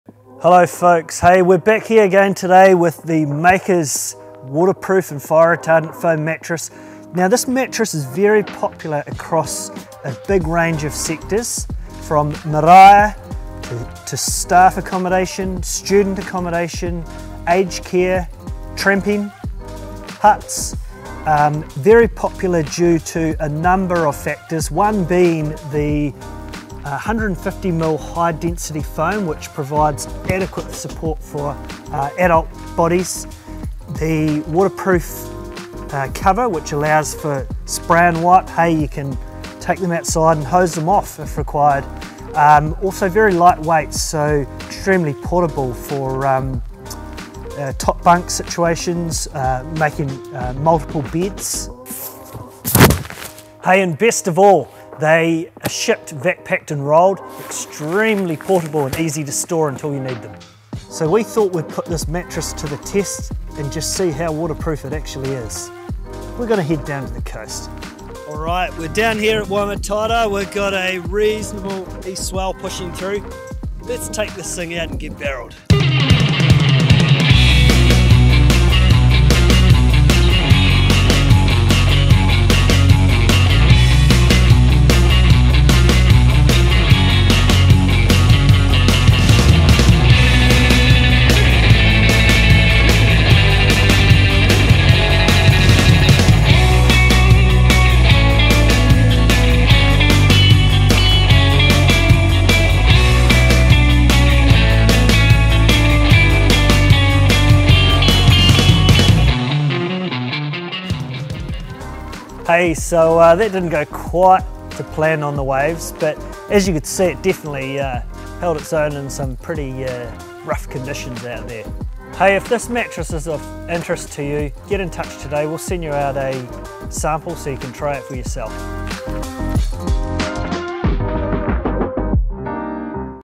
How waterproof is our Makers Waterproof Foam Mattress?
Greg shows off the Makers Foam Mattress and its waterproof cover in style!